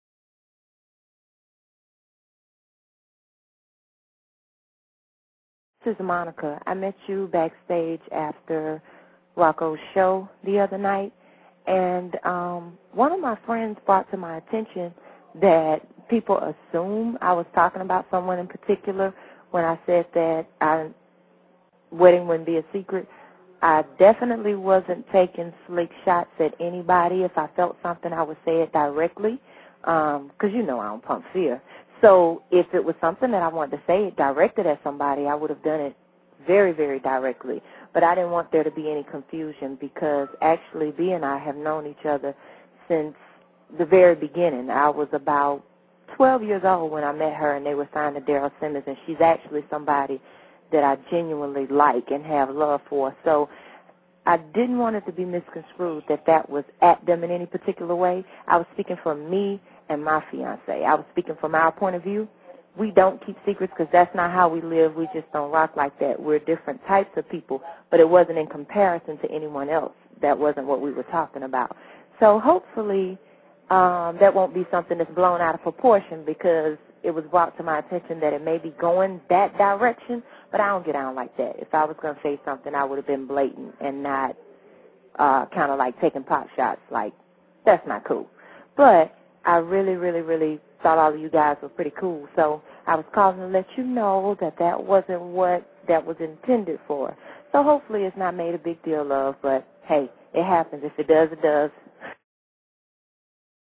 Monica Speaks on Beyonce Beef Rumors
Monica Calls BlogXilla to squash the Rumors about her comments being aimed at Beyonce